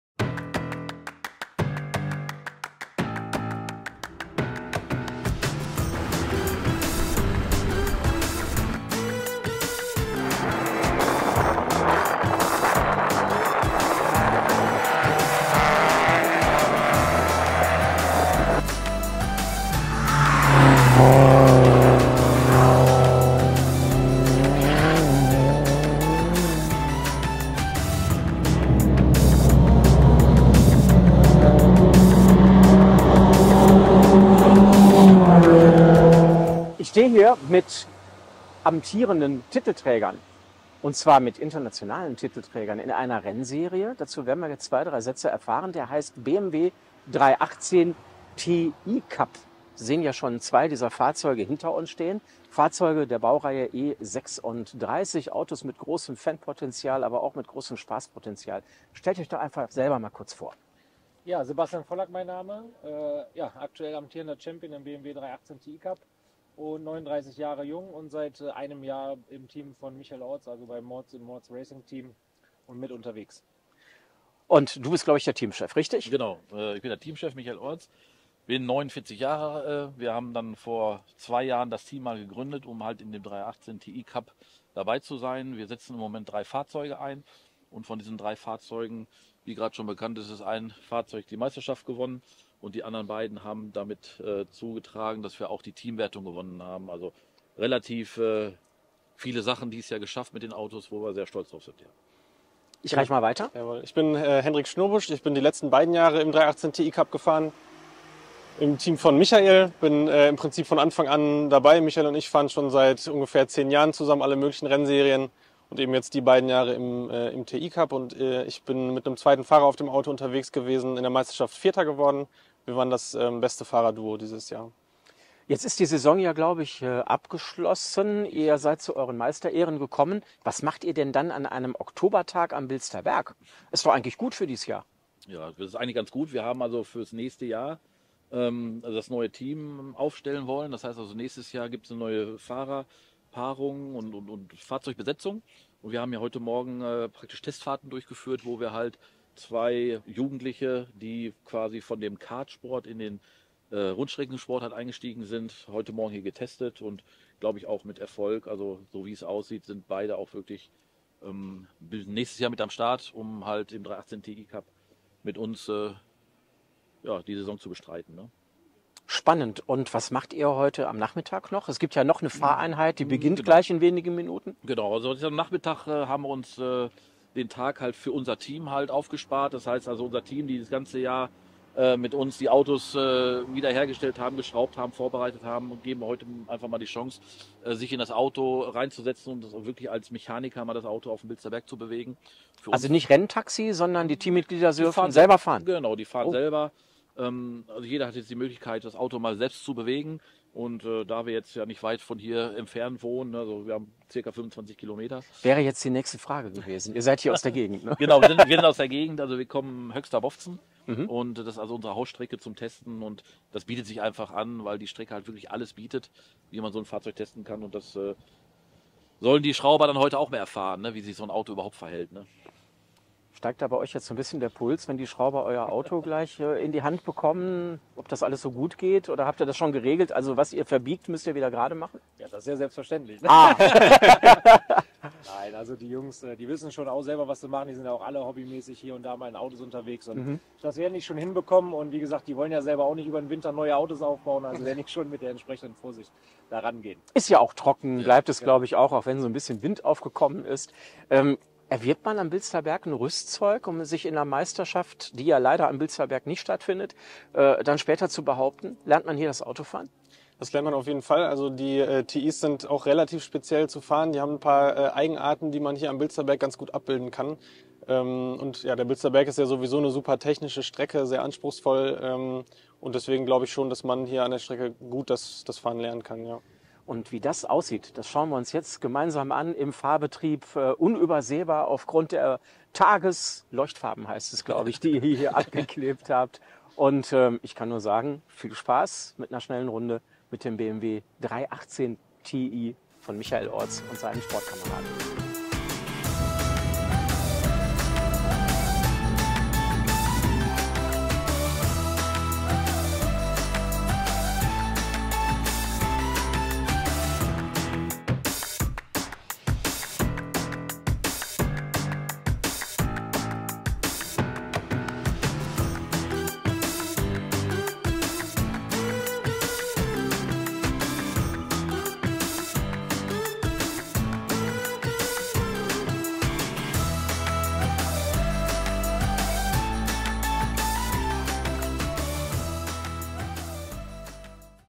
BMW 318 ti Cup - Männer der Stunde - Bergmenschen - BILSTER BERG - Cars and Faces - Episode 01.2023.
Neuer Video-Release auf Carsten Krome Netzwerkeins @YouTube
BMW 318 ti Cup - Männer der Stunde - Bergmenschen - @bilsterberg - Cars and Faces - Episode 01.2023.
Es läuft mehr als gut für das semi-private Rennsportteam von Michael Orts aus Boffzen im Landkreis Holzminden im südlichen Niedersachsen. Der Kraftfahrzeug- und Karosseriebaumeister tritt seit 2021 mit drei farbenfrohen Fahrzeugen im DMV-BMW 318 ti Cup an. Bei dieser Amateur-Rennsportserie müssen sich alle Teilnehmer an einheitliche Regeln halten: An der Fahrwerkskonzeption, der Motor-Getriebe-Einheit sowie dem Reifenmaterial dürfen keinerlei Veränderungen vorgenommen werden. „Vor jeder Rennveranstaltung werden unsere drei Teamfahrzeuge durch einen technischen Kommissar vor Ort überprüft", kommentiert Orts. Auch die Leistung der Motoren muss genauestens den technischen Regularien entsprechen: "148,4 PS sind nach dem Reglement zugelassen", weiß Michael Orts zu berichten. "Wir stellen die Motoren so ein, dass wir stets 1,5 PS unter der Maximalleistung liegen“.
Seit dem vergangenen Wochenende sind Michael Orts und sein schnellster Mann im (Renn-)Stall, der dreifache Meister Sebastian Vollak, in aller Munde. Denn beim diesjährigen Saisonauftakt des @dmvbmw318ticup auf dem Hockenheimring lieferte sich der einstige Rallyepilot ein spektakuläres Duell um den Sieg. Das an sich wäre nicht unbedingt bemerkenswert, wäre der große Gegner nicht ein Gastfahrer mit Formel-1-Vergangenheit gewesen: Timo Glock, zuletzt in der DTM im Einsatz und bei seinem Auftritt hart am Limit unterwegs. Das Zünglein an der Waage mag die Saisonvorbereitung am BILSTER BERG gewesen sein. Dort testet die Truppe von der Weser regelmäßig sowohl Mensch als auch Maschine(n). Europas schönste Test- und Präsentations-Rennstrecke bietet dazu ideale Voraussetzungen.
Das zeigt sich in der neuesten Episode von bilsterberg - carsandfaces, die erste Staffel des noch jungen Jahres 2023. Reporter Carsten Krome stellt authentische Bergmenschen eins zu eins vor, garniert von Fahrszenen, die auf den 4.2 Kilometern im Teutoburger Wald entstanden sind: ein Fest am Mittwochabend, nicht nur für Fans des mit 50 Startern top-besetzten Cups.